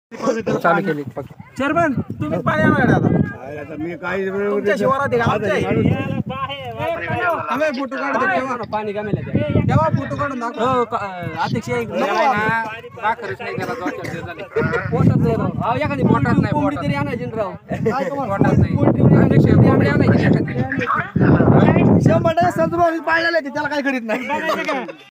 LEOPARD RESCUE
Leopard rescued but looks totally exhausted & traumatised... Cheers to the fearless Rescue team. Excellent work...God Bless